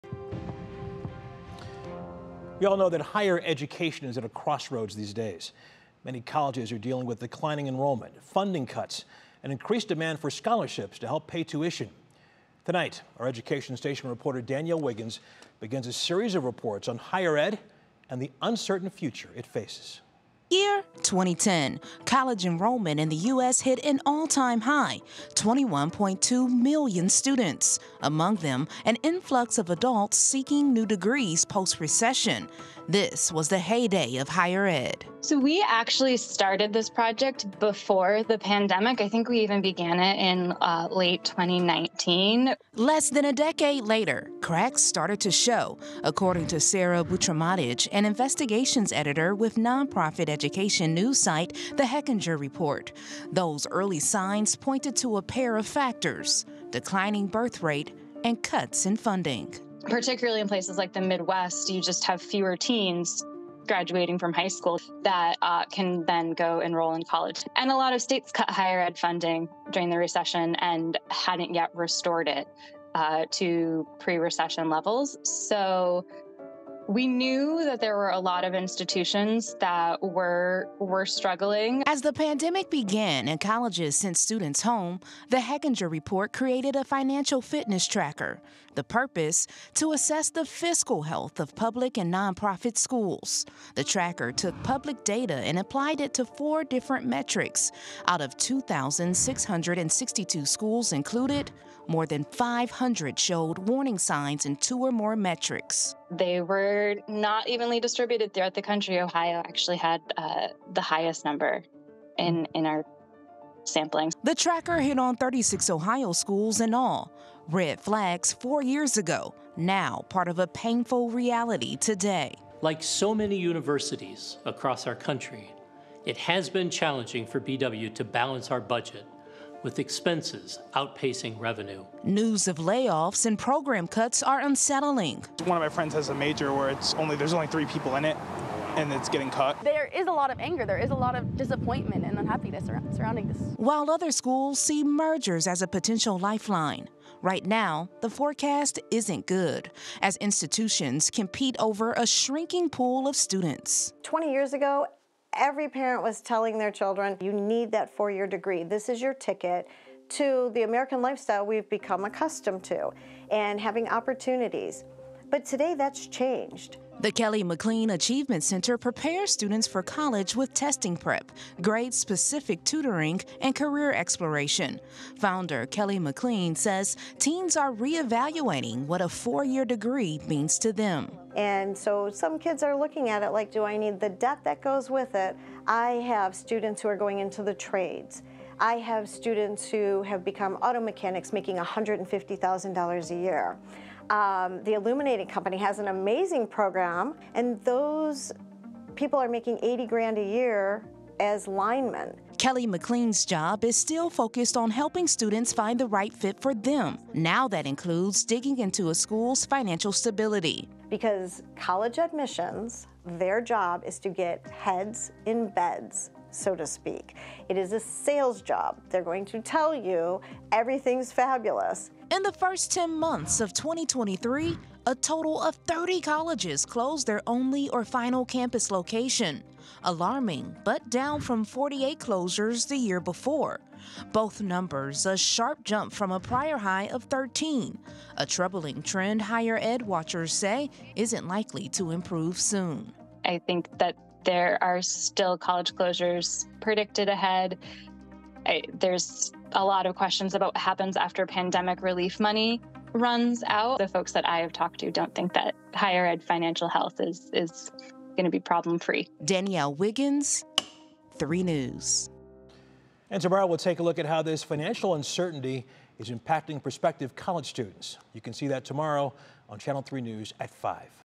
Higher Ed: Uncertain Future | The financial reckoning facing colleges and universities
Decades ago, the message was clear: Earning a college degree was the best pathway to financial success.

Today, the future of higher education has never been more uncertain. Enrollment has declined, schools are cutting staff and programs, and institutions are even closing.

It’s a far cry from 2010, when college enrollment in the United States hit an all-time high. Roughly 21.2 million students filled campuses, among them an influx of adults seeking new degrees post-recession. Expansion projects also brought bigger buildings, technology, and programs.

This was the heyday of higher ed, but things have changed.

“We actually started this project before the pandemic. I think we began it in late 2019.”

3News reached out to Sarah Butrymowicz, investigations editor with nonprofit education news site The Hechinger Report. In 2020, its series "Colleges in Crisis" detailed the financial stress facing a growing number of schools. Early signs pointed to a pair of factors: a declining birth rate and cuts in funding.

"We actually started this project before the pandemic — I think we began it in late 2019," Butrymowicz remembered. "Particularly in places like the Midwest, you have fewer teens graduating from high school — just fewer teens overall — that can go enroll in college, and a lot of states cut higher ed funding during the recession and hadn't restored it to pre-recession levels. So we knew that there were a lot of institutions that were struggling."

As the pandemic began and colleges sent students home, The Hechinger Report created a financial fitness tracker, the purpose being to assess the fiscal health of public and nonprofit schools.

The tracker took public data and applied it to four different metrics: enrollment, retention, average tuition, and endowment/costs. Out of 2,662 schools included, more than 500 showed warning signs in two or more metrics.

"And they were not evenly distributed throughout the country," Butrymowicz explained. "Ohio had the highest number in our sampling."

The tracker identified 36 Ohio schools with red flags four years ago. Today, college financial issues are a part of our state's reality, with Baldwin Wallace University, Notre Dame College, Lake Erie College, Lakeland Community College, and Miami University all in the news recently over their budget struggles.

As a whole, the forecast for higher education isn't good, as schools compete over a shrinking pool of students.

Danielle Wiggins reports. --